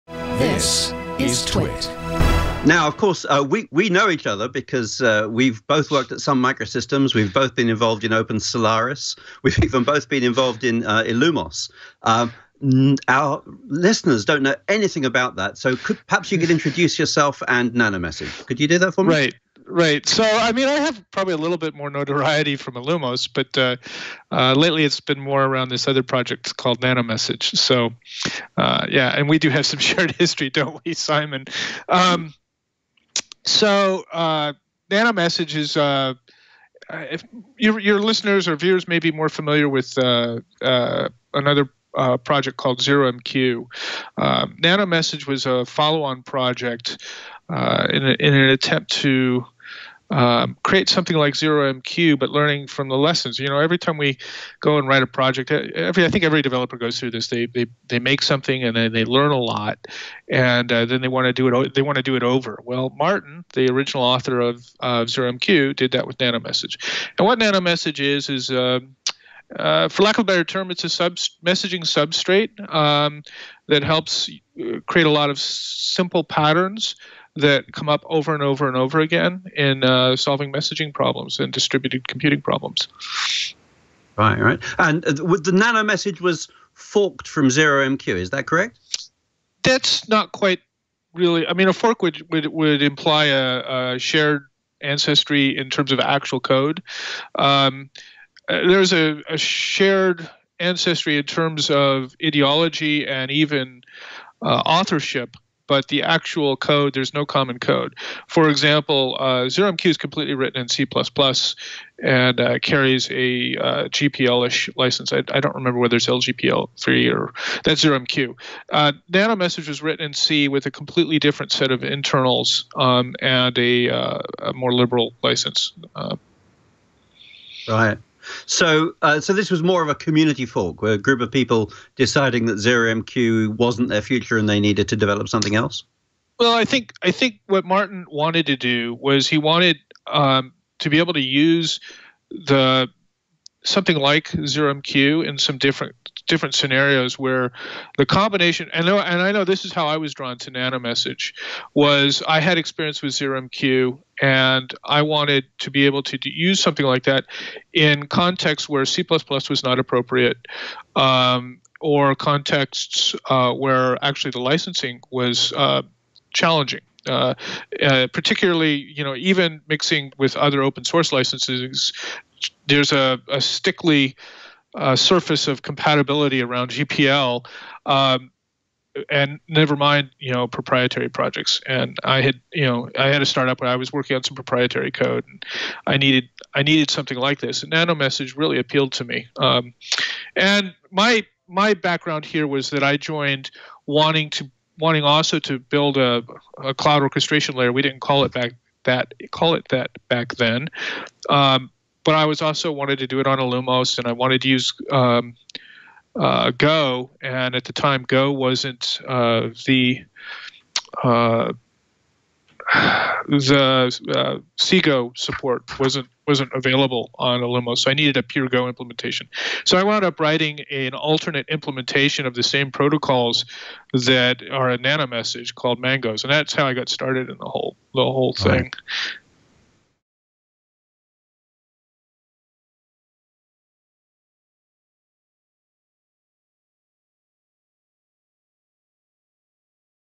nanomsg Overview
nanomsg is a is a socket library that provides several common communication patterns. It aims to make the networking layer fast, scalable, and easy to use.
Garrett worked for Sun on the legendary E10K, but also did crypto work, drivers, and ultimately served on the architectural committee for Solaris.